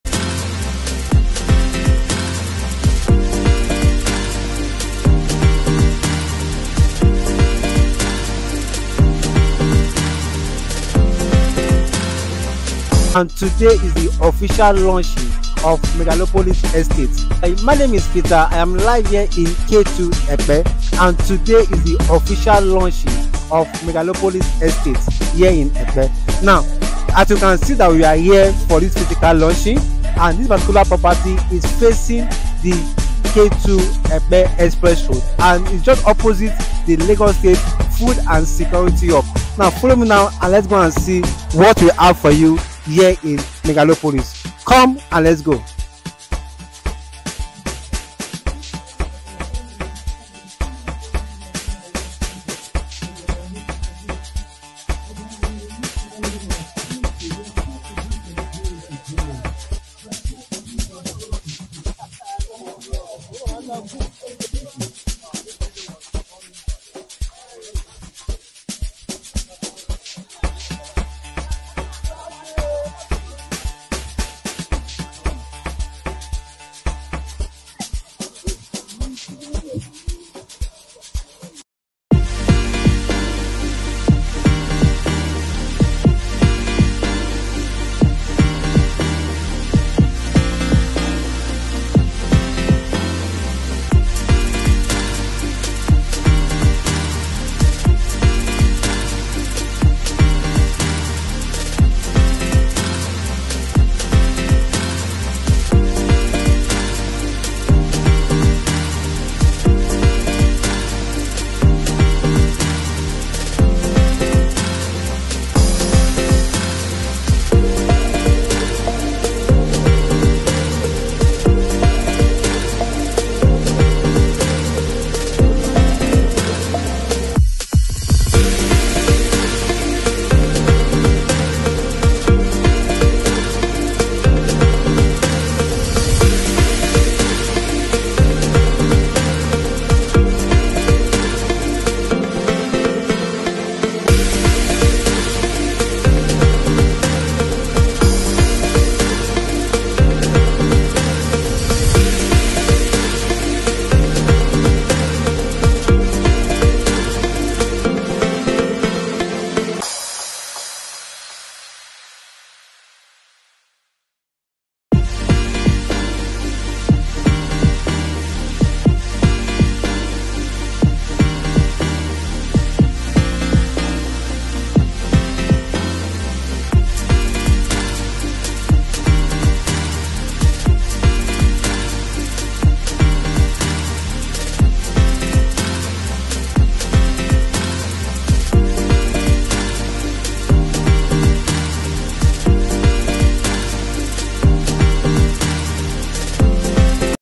OFFICIAL LAUNCHING OF MEGALOPOLIS ESTATE EPE
Official launching of megalopolis estate

Here is an official launch of megalopolis estate

Megalopolis was launched yesterday, Tuesday, 24th October 2023

The launch price for megalopolis is N7M for 500sqm and N4.5M for 300sqm

Initial deposit is N1M

Megalopolis estate face the road directly and it is directly opposite the Lagos State Food Security and Logistics hub

The best time to key into megalopolis estate Epe is now

Next price is N15M for 500sqm. Why not buy for N7M before the next price of N15M